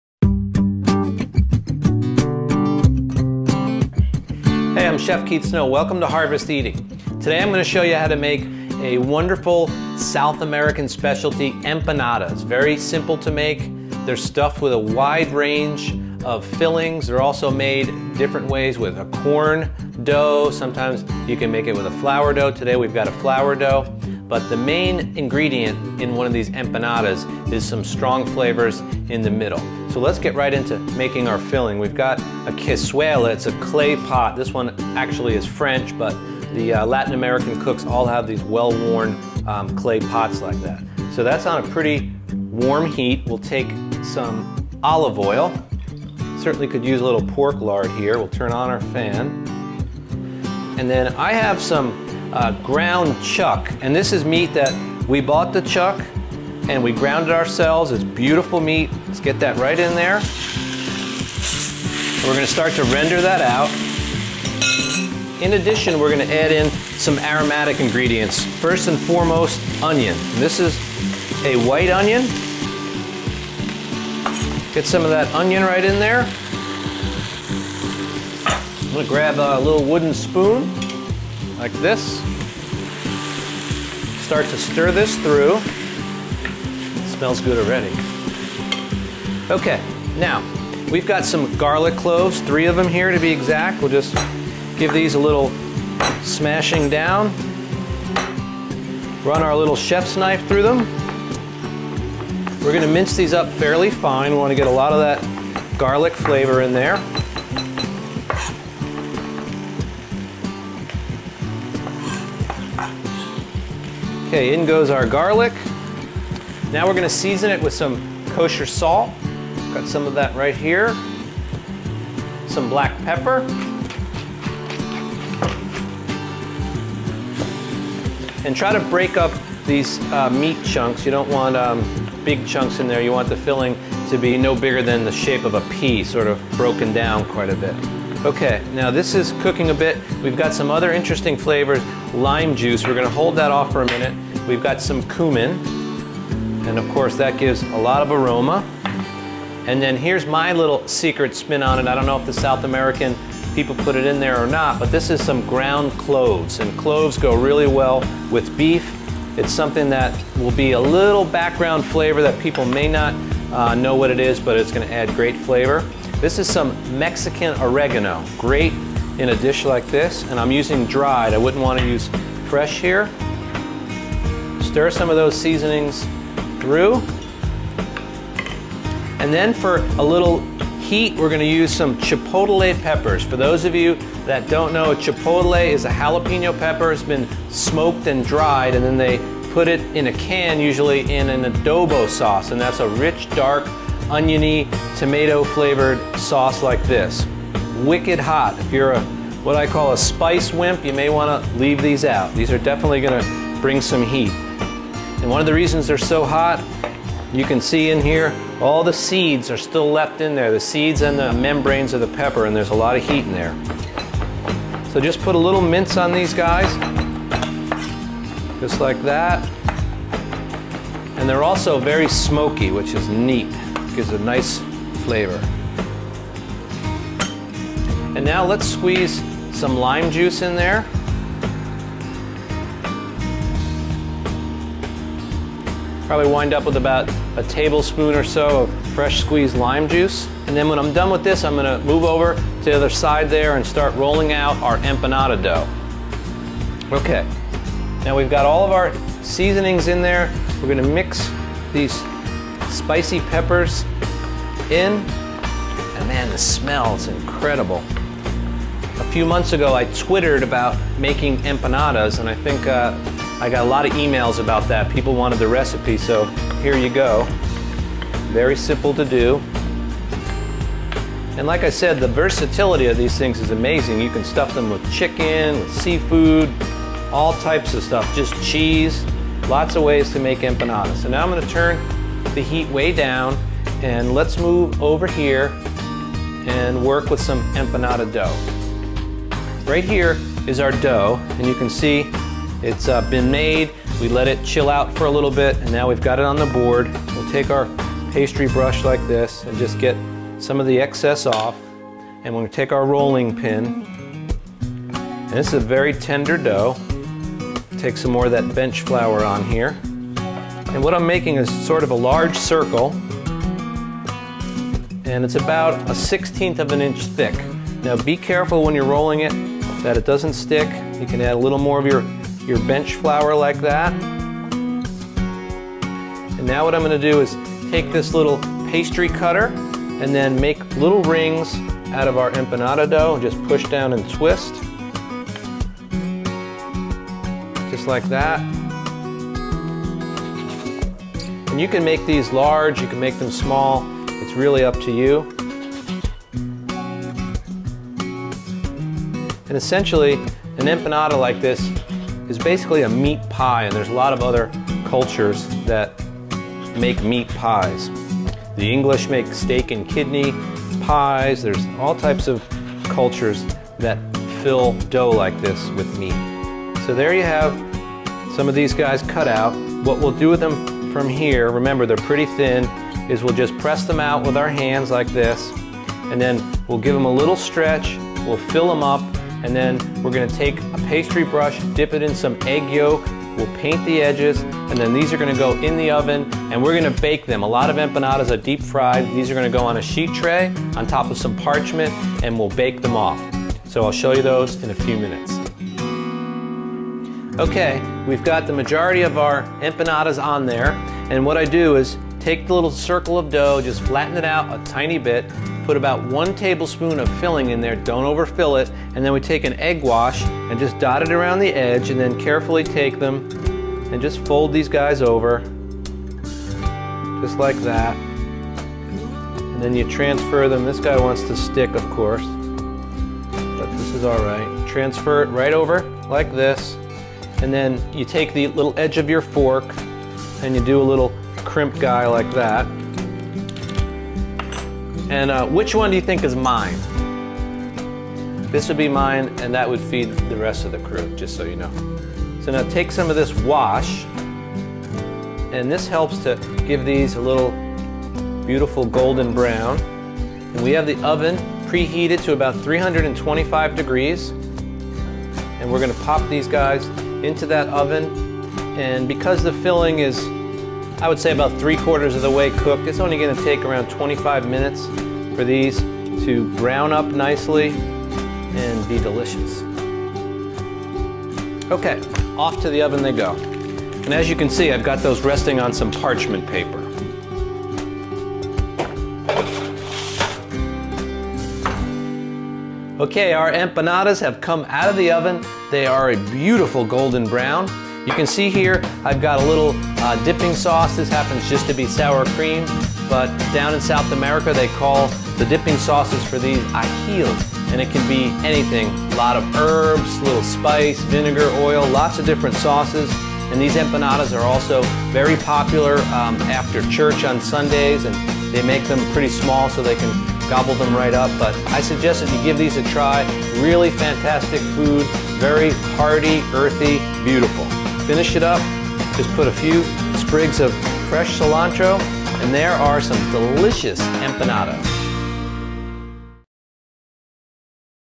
Chipotle Beef Empanadas
This simple empanadas are very simple to make and very delicious. They can be made ahead and frozen successfully. The dough is very simple and easy to roll out because it contains lard which is very classic in Latin america.